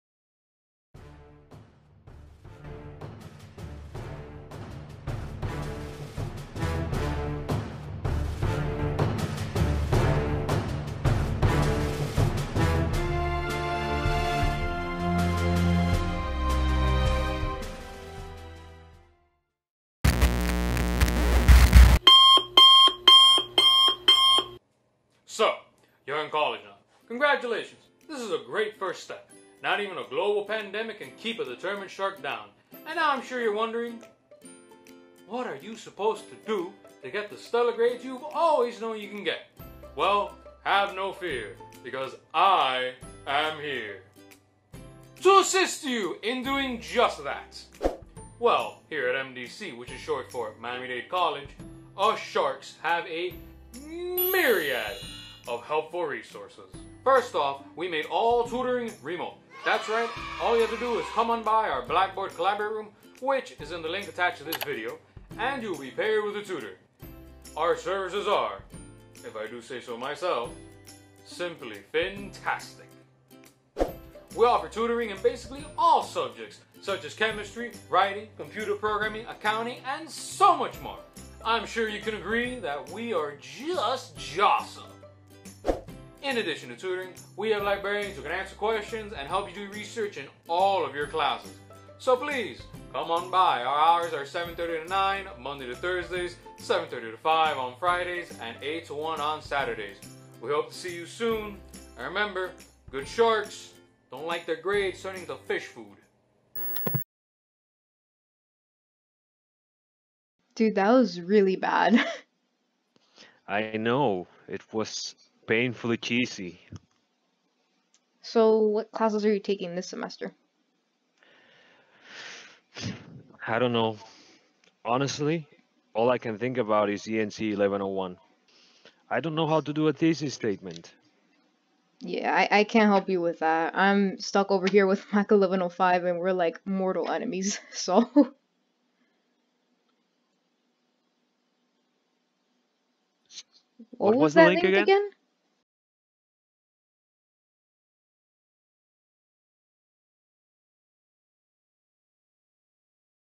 Remote Tutoring - MDC Learning Resources
Learning Resources welcomes Miami Dade College students to a brand new Fall term. Our tutors and librarians are ready to assemble and help you succeed!